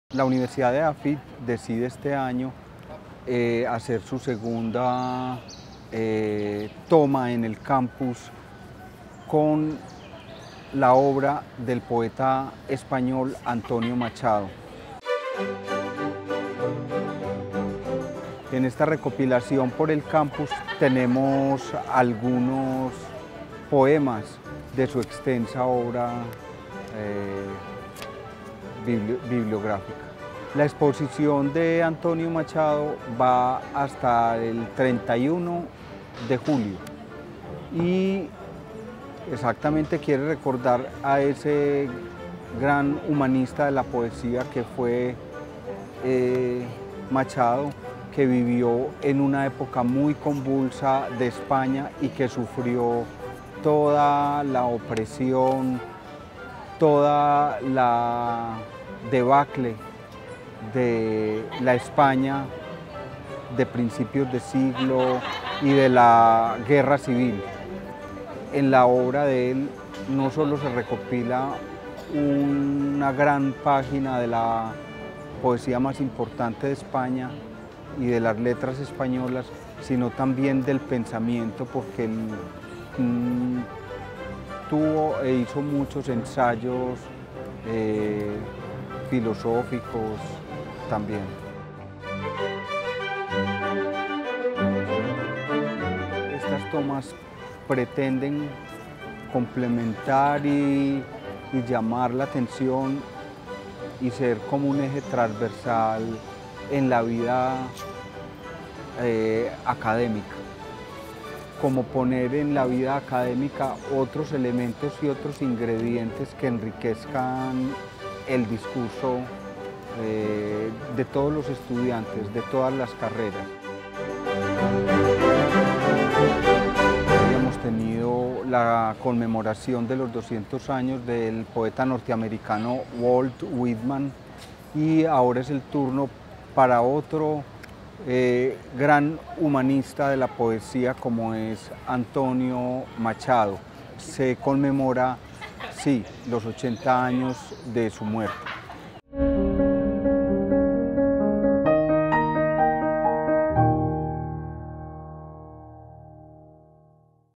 Al paso de los eafitenses aparecen los versos románticos y humanistas de Antonio Machado
• Con motivo de los 80 años de la muerte del poeta español, la Institución hizo, en esta segunda presencia en el campus del año, una recopilación de sus más destacados escritos, con el fin de enriquecer la vida académica y cultural de la comunidad universitaria. La exposición estará a la vista hasta el 31 de julio.

• Proverbios y Cantares cuenta con 22 artes, distribuidos en 11 estructuras, que aparecen al paso de los eafitenses cerca de lugares como la Librería, Bancolombia, la portería de Las Vegas, el Auditorio Fundadores, la Biblioteca y el bloque 38, para mostrar la inspiración de este poeta humanista que murió en el exilio al final de la Segunda República Española.